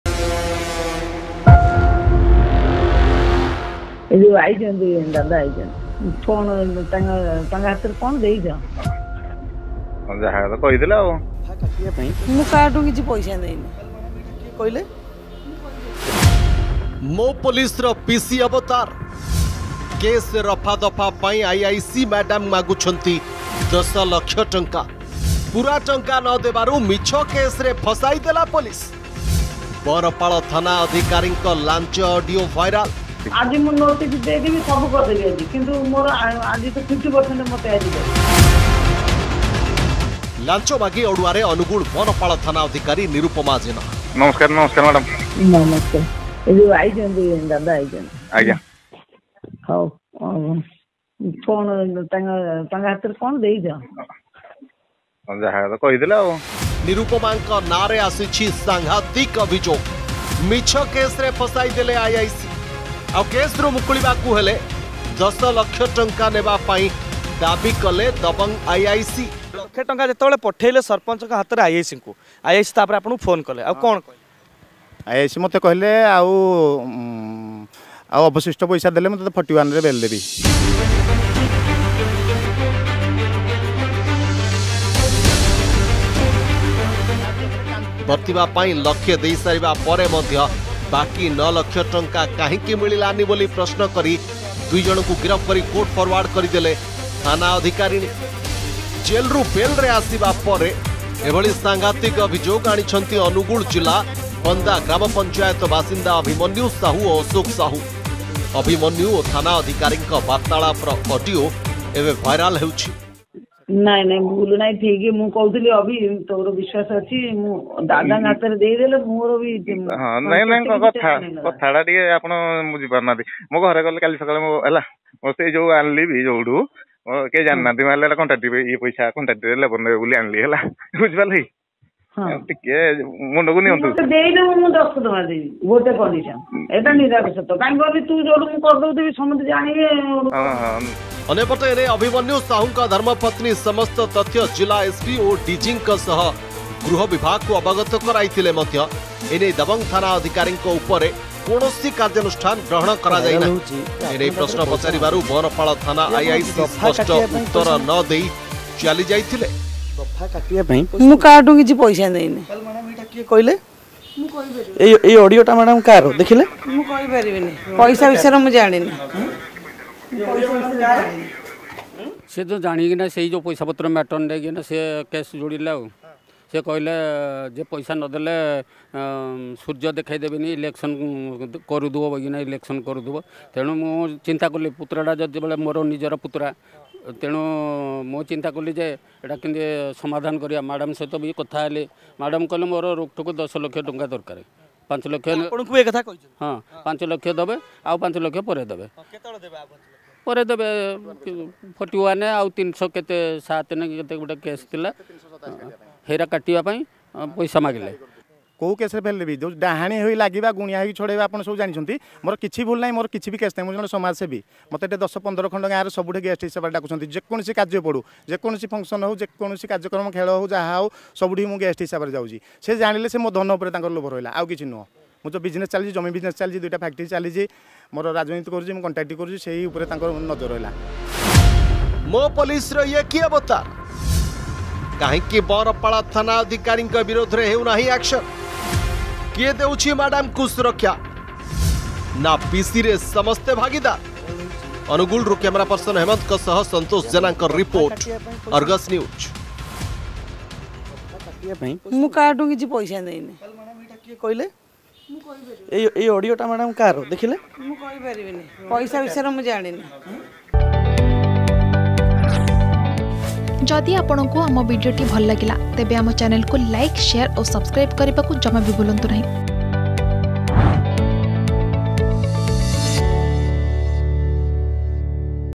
ମୋ ପୋଲିସର ପିସି ଅବତାର || Nirupama Jena || Bribery Case || Angul
ମୋ ପୋଲିସର ପିସି ଅବତାର । କେଶ ରଫାଦଫା ପାଇଁ ଆଇଆଇସି ମ୍ୟାଡମ୍ ମାଗୁଛନ୍ତି ୧୦ ଲକ୍ଷ । ପୁରା ଟଙ୍କା ନ ଦେବାରୁ ମିଛ କେସରେ ଫସାଇଦେଲା ପୋଲିସ । ବଅଁରପାଳ ଥାନା ଅଧିକାରୀଙ୍କ ଲାଞ୍ଚ ଅଡିଓ ଭାଇରାଲ୍ ।